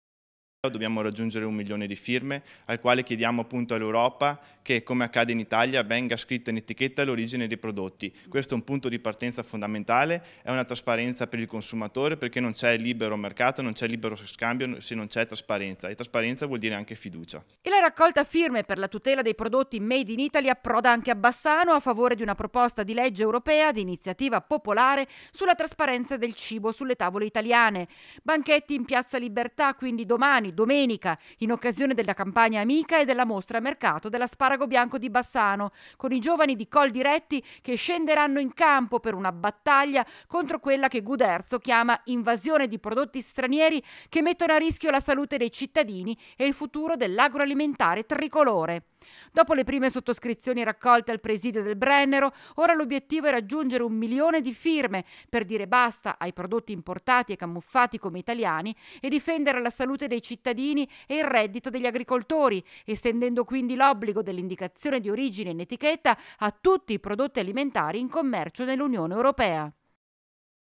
DOMENICA GIOVANI COLDIRETTI IN PIAZZA PER RACCOGLIERE FIRME A TUTELA DEL MADE IN ITALY | 20/04/2024
TG Bassano del Grappa 20/04/2024 - BASSANO DEL GRAPPA - Domani banchetti dei giovani di coldiretti in piazza a bassano per raccogliere le firme da portare in Europa per la tutela del made in Italy || La raccolta firme per la tutela dei prodotti made in Italy approda anche a Bassano, a favore di una proposta di legge europea di iniziativa popolare sulla trasparenza del cibo sulle tavole italiane. Banchetti in piazza Libertà, domani, domenica, in occasione di campagna amica e della mostra mercato dell'asparago bianco di Bassano, con i giovani di Coldiretti che scenderanno in campo per una battaglia contro quella che Guderzo chiama "invasione di prodotti stranieri che mettono a rischio la salute dei cittadini e il futuro dell'agroalimentare tricolore". Dopo le prime sottoscrizioni raccolte al presidio del Brennero, ora l'obiettivo è raggiungere un milione di firme per dire basta ai prodotti importati e camuffati come italiani e difendere la salute dei cittadini e il reddito degli agricoltori, estendendo l'obbligo dell'indicazione di origine in etichetta a tutti i prodotti alimentari in commercio nell'Ue. - Intervistati PIETRO GUDERZO (Presidente Coldiretti Vicenza) (Servizio di Vanessa Banzato) - Segui Rete Veneta anche sul digitale terrestre! Visita il sito